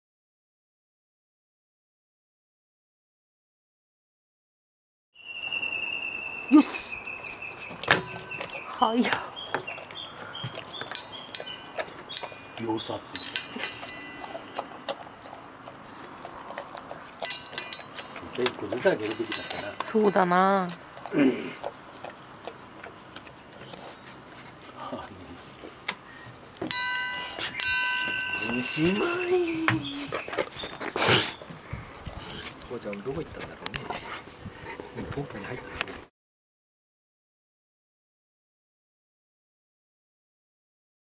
BerneseMountainDog eats a cake
同居犬バーニーズマウンテンドッグのハンフリー君の４歳のお誕生日に、犬用バナナケーキを焼いたので、お裾分けしてもらった幸太郎君。あっという間に完食でした。